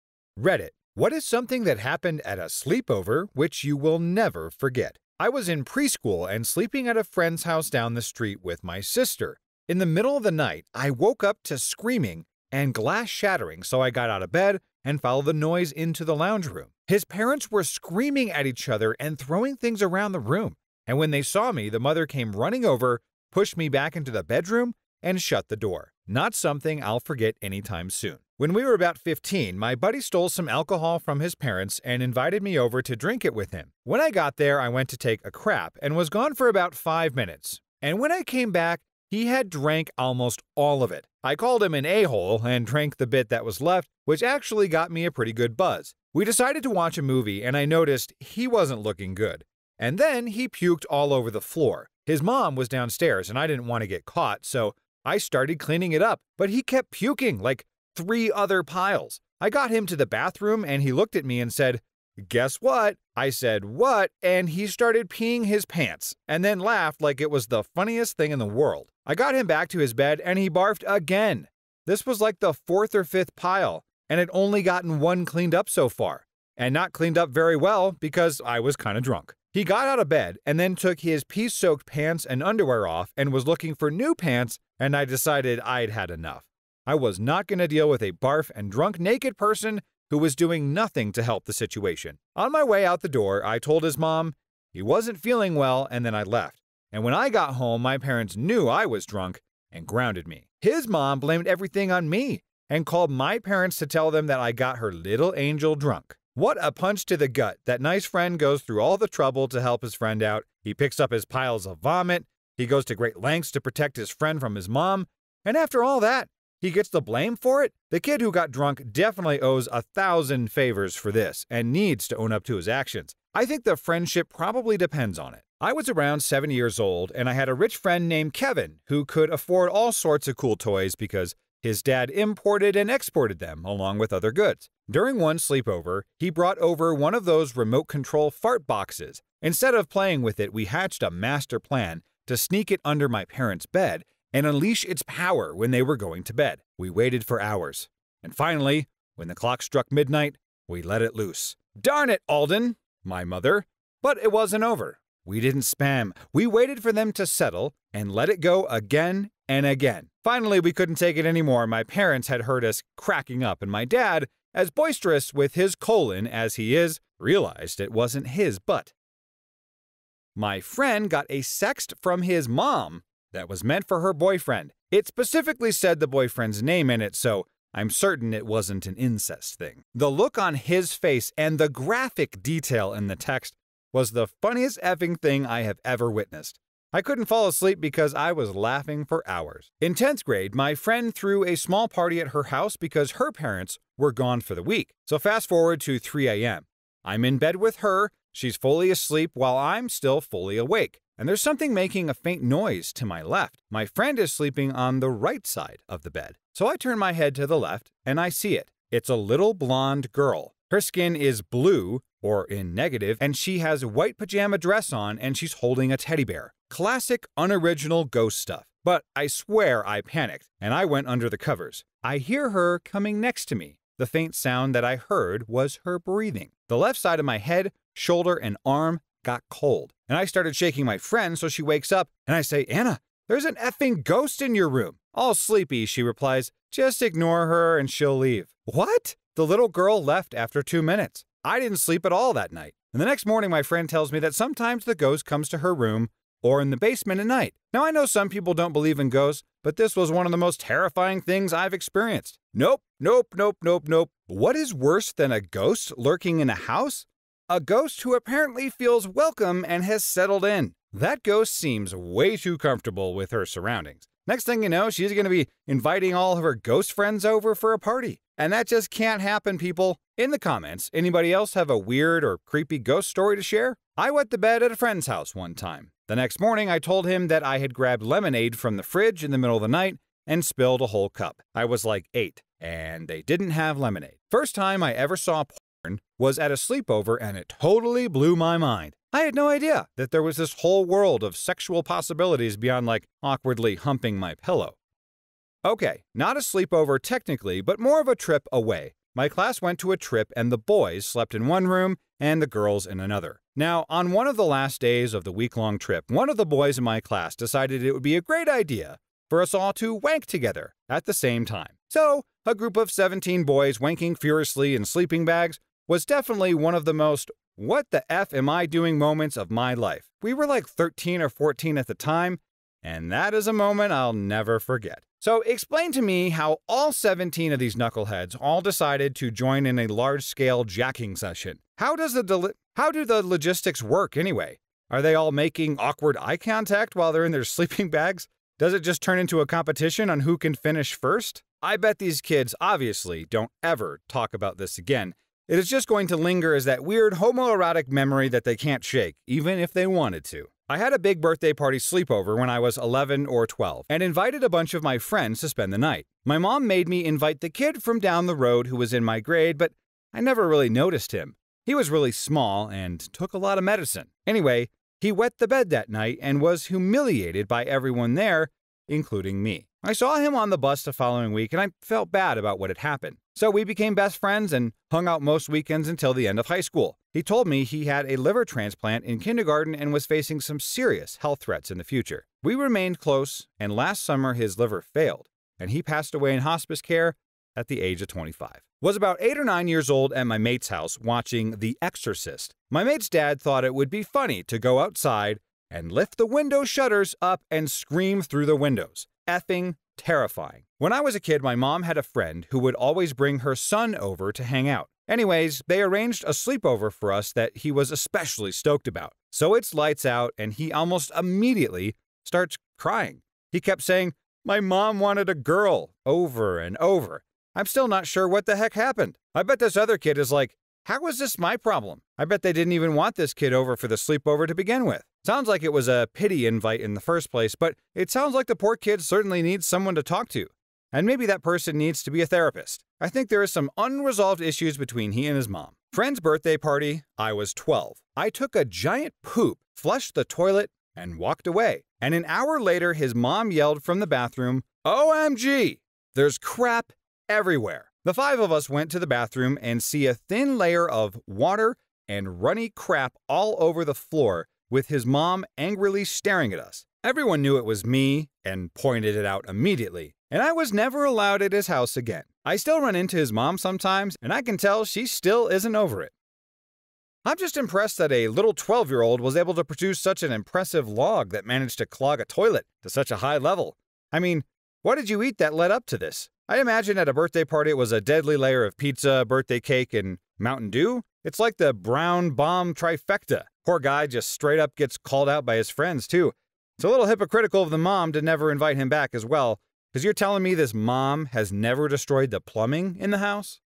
What is something that happened at a sleepover you will never forget?
This video is filled with crazy stories. Be sure to watch the entire video ;)